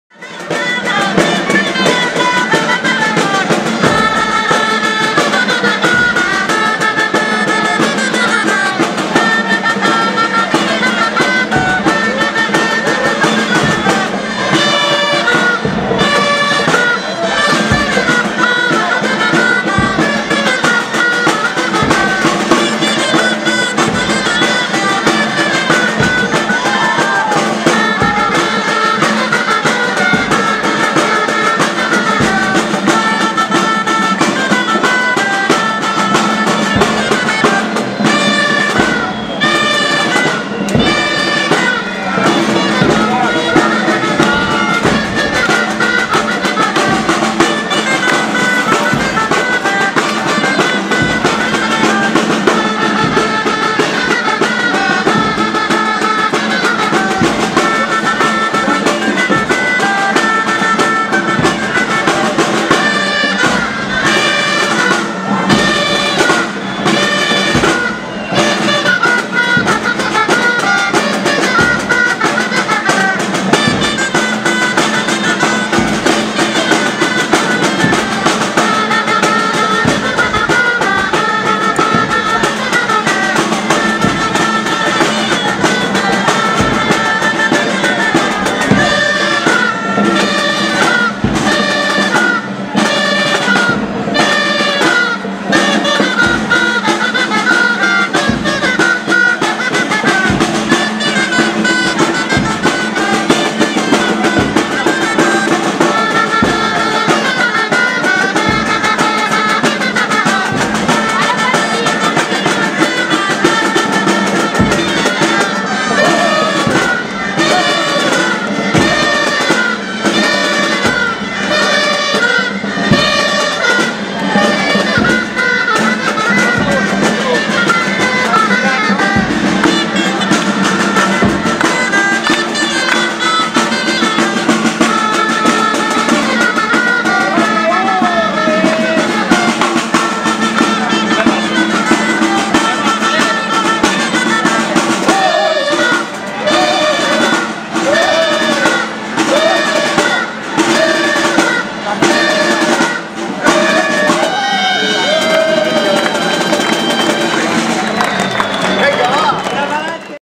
Festa Major Creu Alta 2010 - " Els Nuvis de la Creu Alta "
Ballada dels Gegants de la Creu Alta i de la Concòrdia.